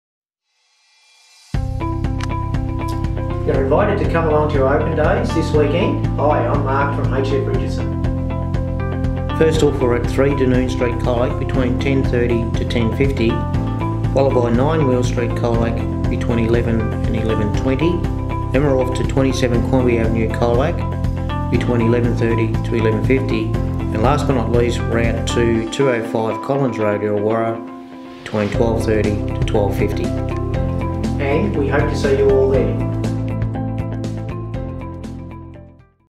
HF Richardson's Open For Inspection 31 October 2015
Just the four opens this week but they are great ones - Check out 205 Collins Road Irrewarra & 3 Dunoon Street Colac !